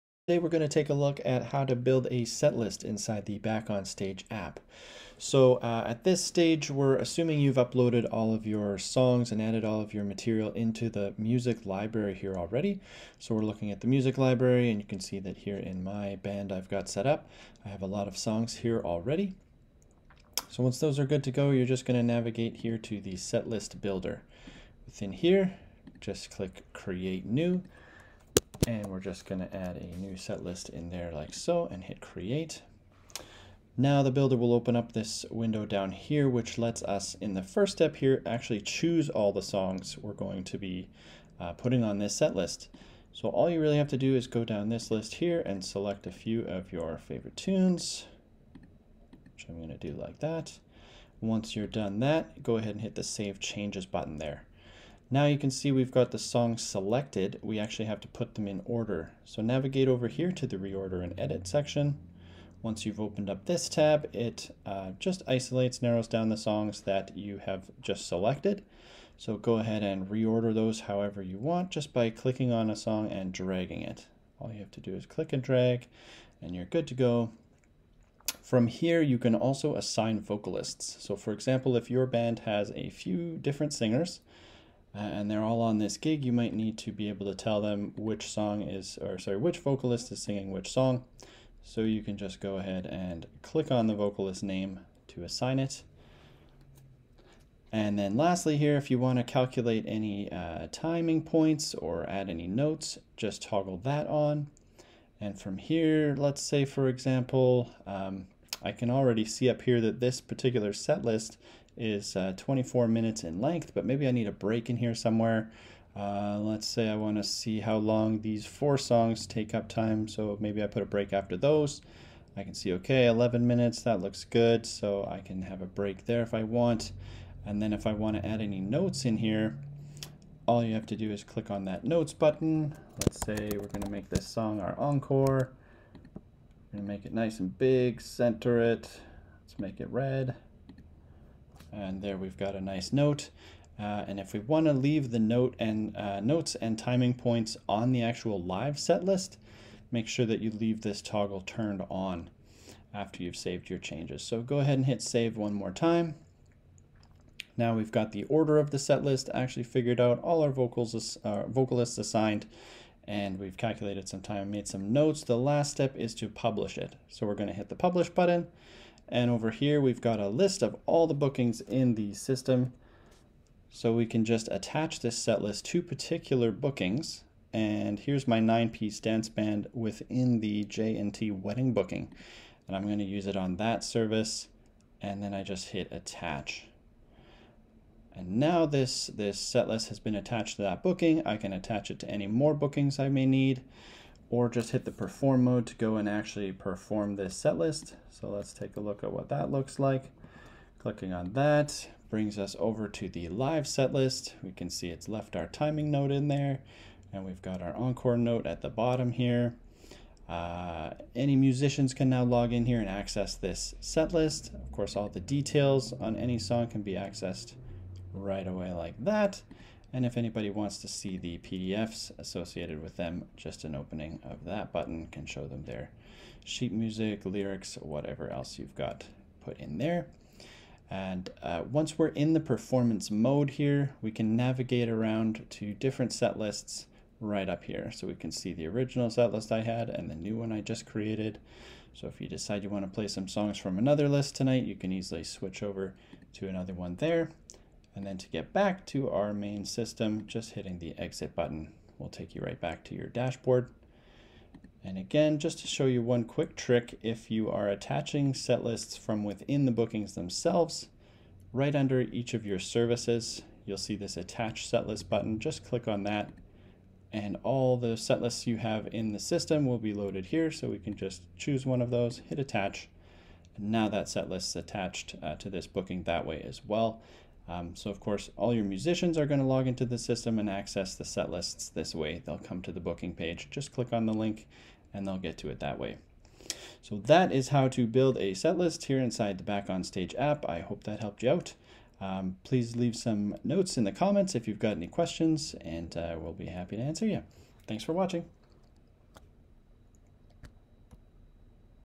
How To Build A Set List In Seconds with Back On Stage Band App
You can build a set list in seconds with the Back On Stage band management app. Watch this video to learn how.